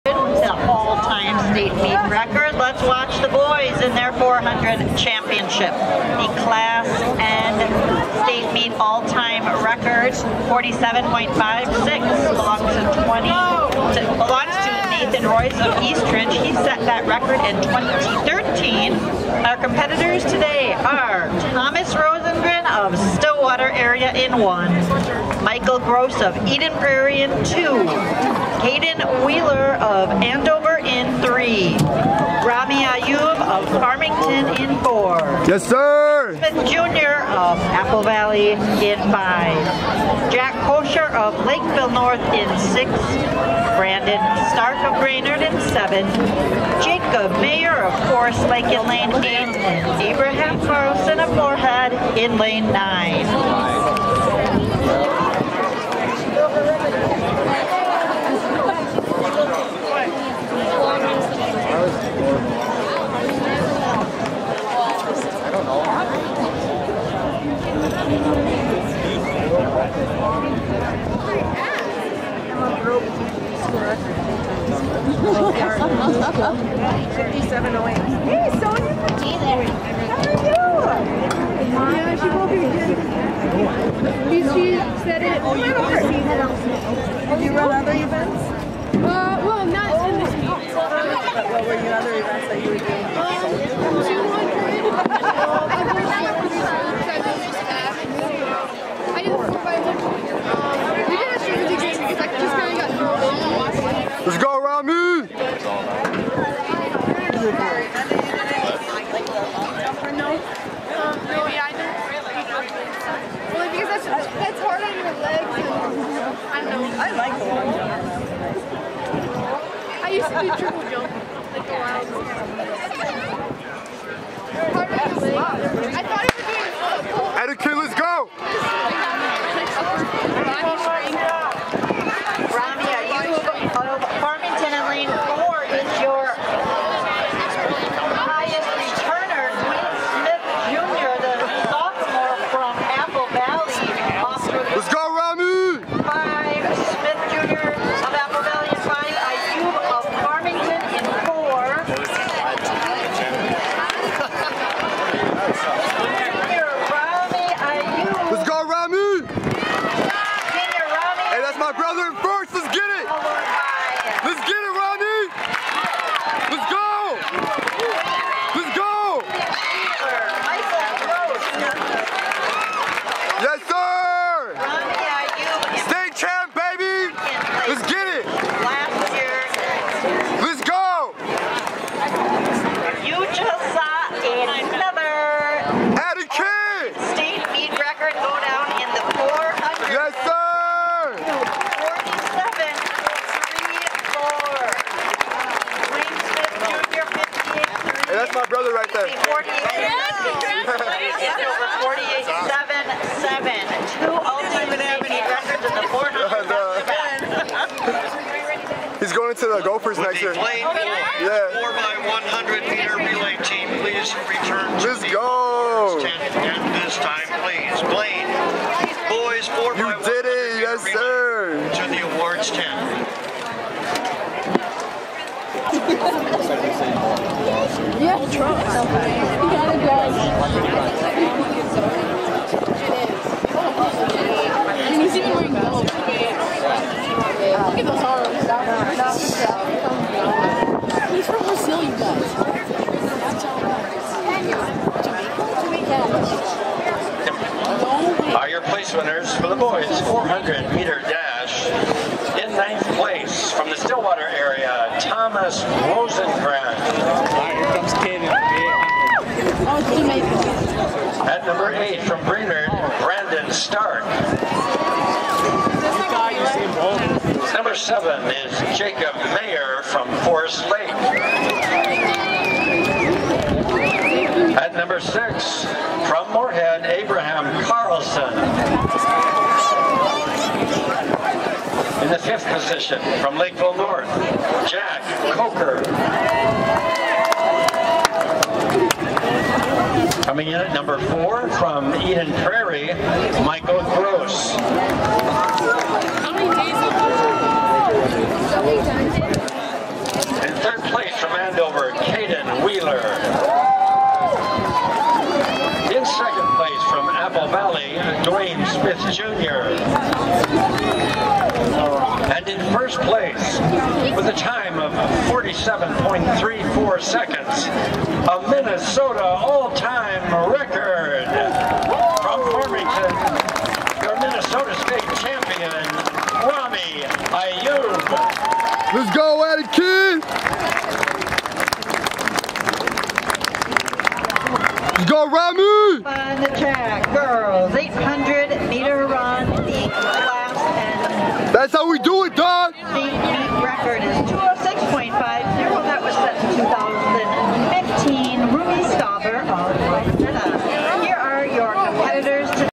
THE ALL TIME 400M MN STATE RECORD GETS BROKEN BY RAMY AYOUB'S 47.34 RACE WHILE BECOMING STATE CHAMP!
Ramy Ayoub comes back this State Meet winning his 400m breaking the MN All Time and State Meet Record with a time of 47.34 after finishing as a runner-up last year. Ramy Ayoub finishes his highschool career a MN ALL TIME GREAT, dominating the 400 and 800 with the fastest posted times two years in a row, which is the fastest times in more than 20 states.

Boys 400 Meter Dash Class AAA MN State Meet
1 Ramy Ayoub                12 Farmington                47.34C  - State Meet and MN All Time Record
2 Dwyne Smith Jr.          10 Apple Valley              48.33
3 Caden Wheeler            12 Andover                     48.77
4 Michael Gross             11 Eden Prairie              49.02
5 Jack Kocher                 12 Lakeville North        49.05
6 Abraham Carlson        11 Moorhead                 49.76
7 Jacob Mayer                11 Forest Lake              49.91
8 Brandon Stark             10 Brainerd                     50.11
9 Thomas Rosengren     12 Stillwater Area        50.94